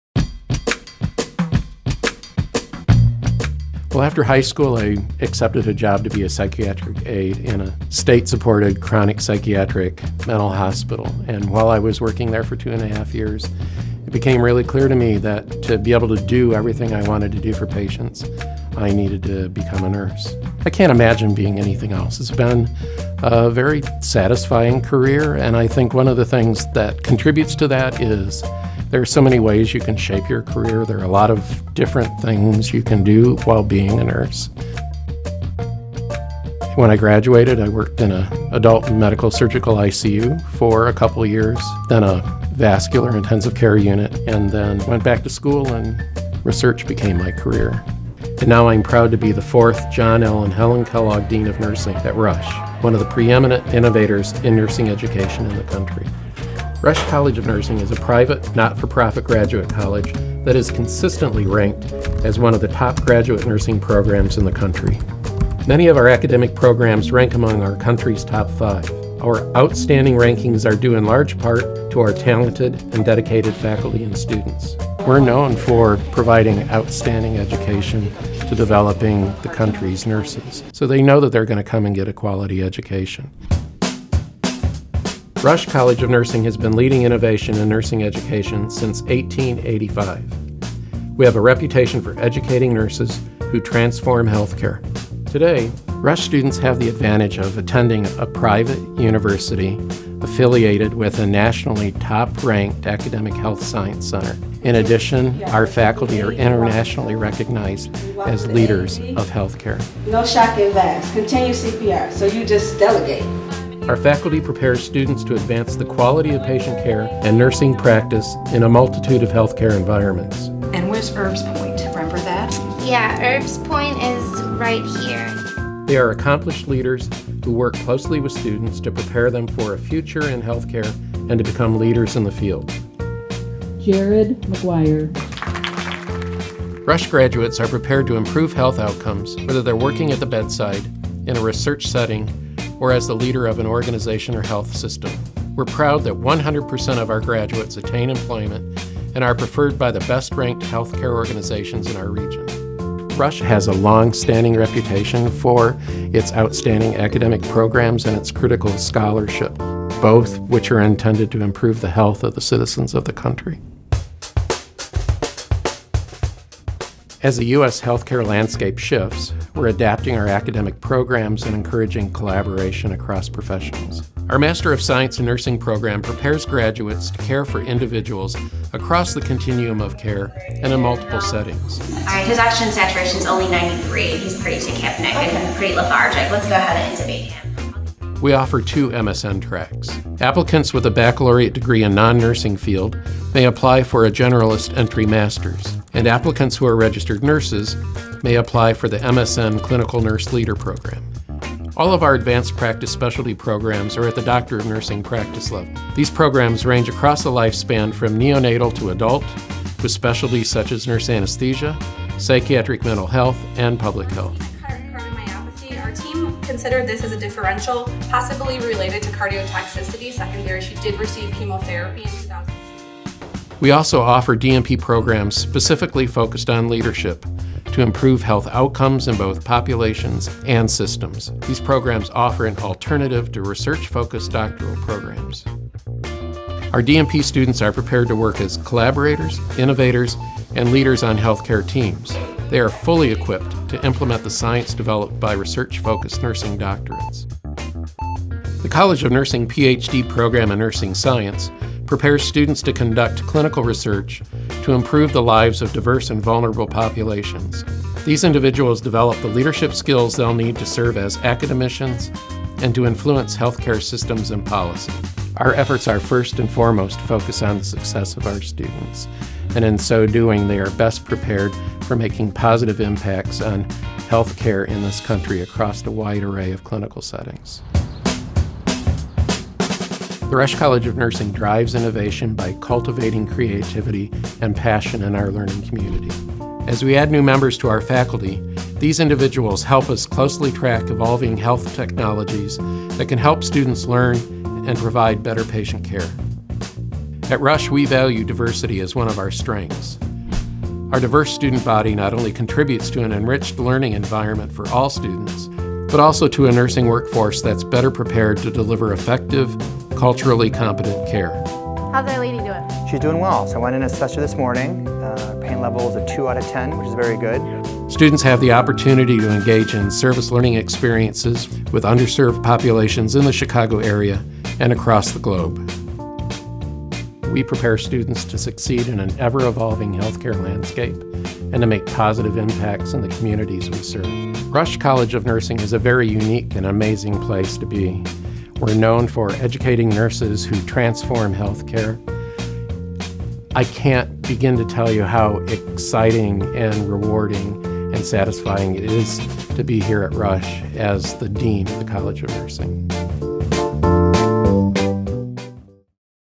Rush University College of Nursing Overview
Marquis Foreman, PhD, RN, FAAN, dean of Rush University's College of Nursing, introduces himself and talks about how Rush helps students succeed in an evolving health care landscape.